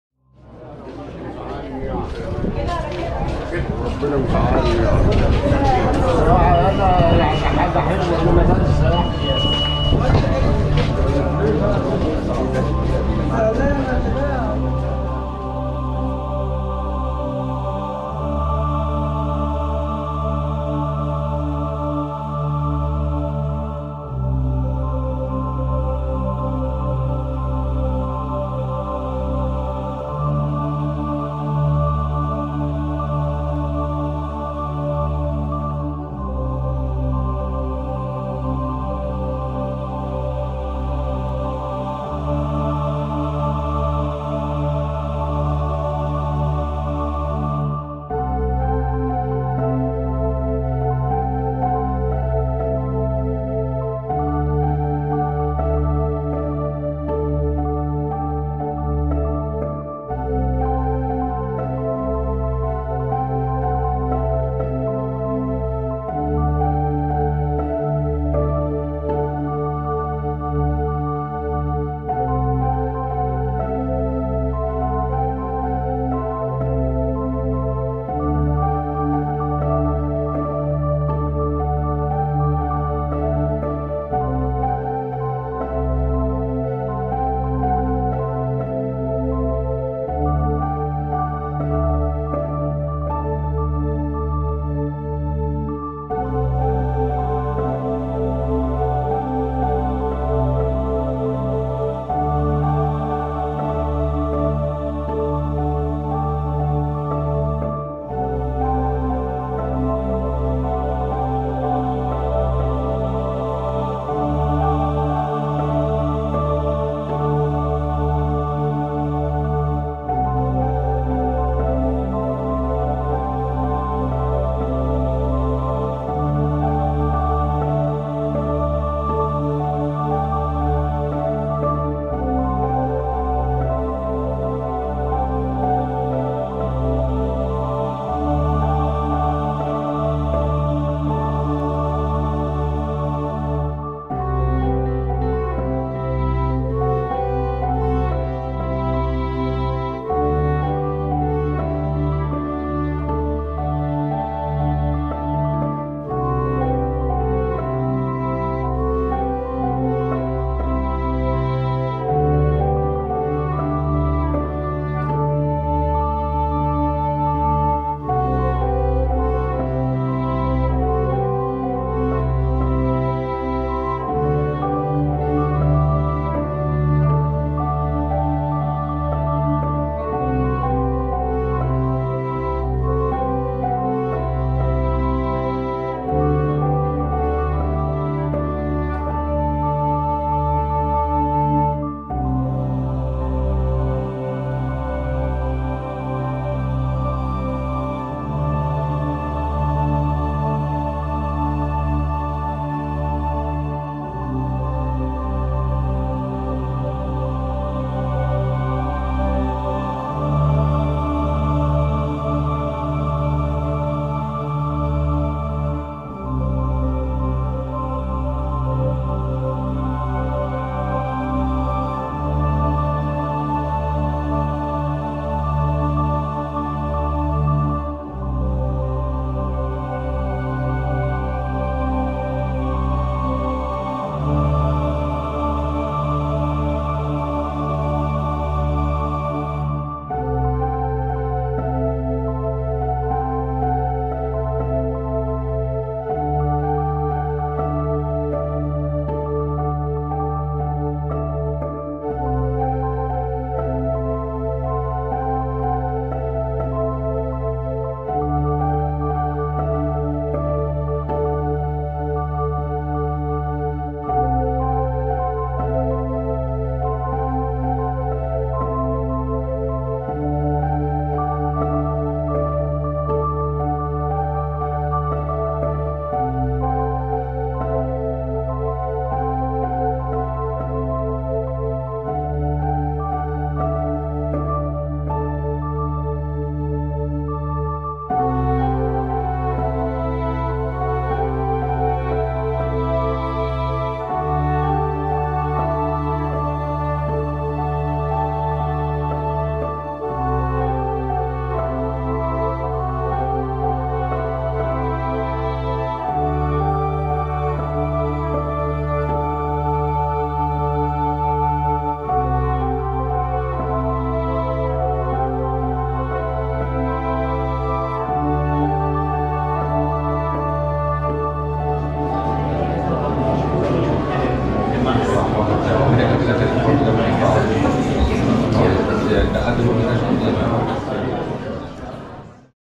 Église Saints-Serge-et-Bacchus, L'endroit où la Sainte Famille aurait vécu, Caire, Égypte
L'église Saints Serge et Bacchus, également connue sous le nom d'Abou Serga, au Caire copte est l'une des plus anciennes églises coptes d'Égypte, datant du 4ème siècle.

On pense traditionnellement que l'église Saints Serge et Bacchus a été construite à l'endroit où la Sainte Famille, Joseph, Marie et l'enfant Jésus-Christ, se sont reposés à la fin de leur voyage en Égypte. Ils ont peut-être vécu ici pendant que Joseph travaillait à la forteresse.
 Wikipédia (anglais)